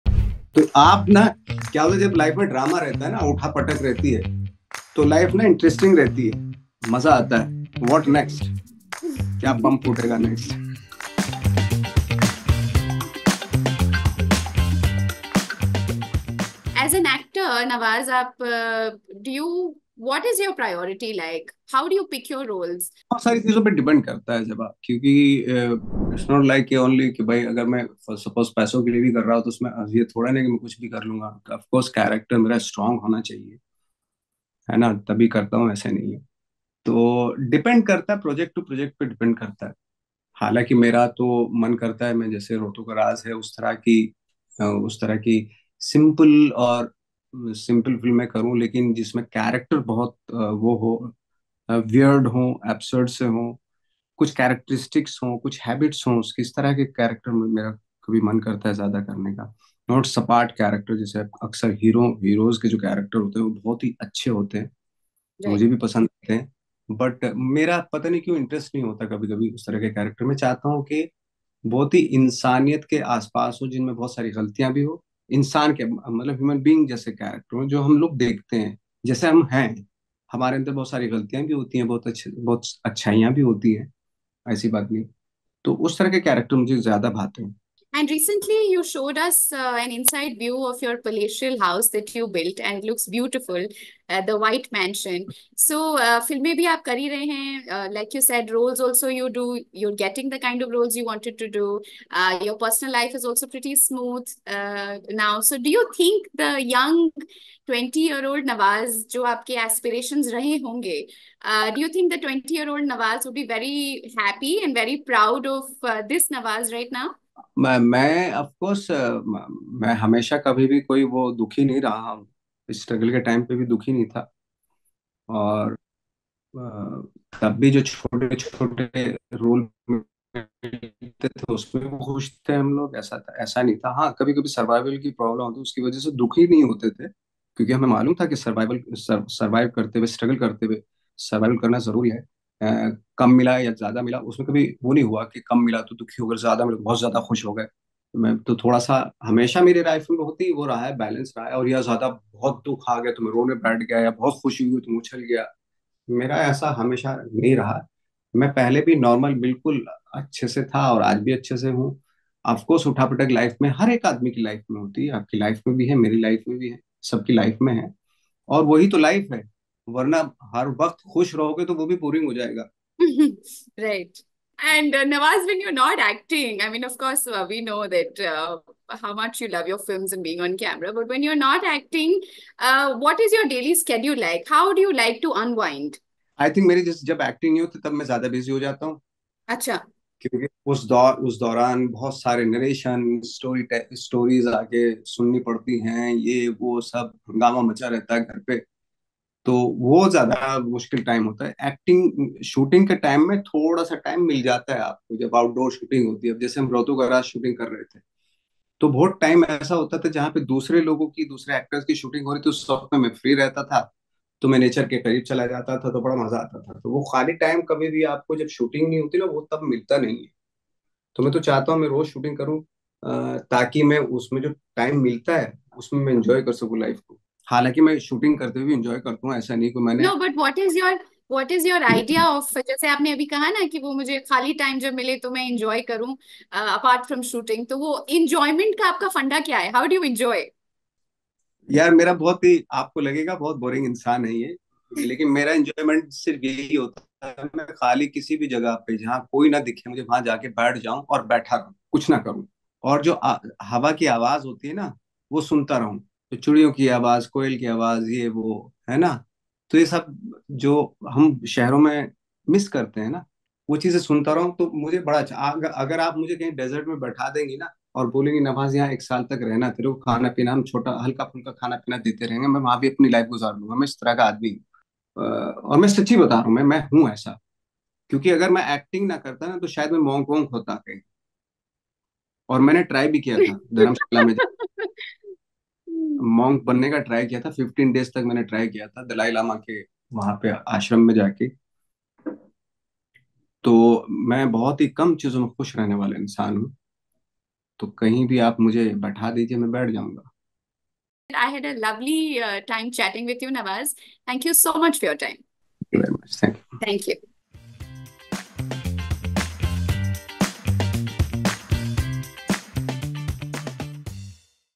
Nawazuddin Siddiqui loves drama in his characters and his life: Interview | WION Exclusive
Nawazuddin Siddiqui owns every character and makes it truly his own. Recently seen in the ZEE5 Global show Rautu Ka Raaz, Nawaz spoke to WION’s Zeba Khan about how he chooses his projects now, reveals he would have been a monk if not an actor and if 20-year-old Nawaz would be happy seeing where he has reached in life.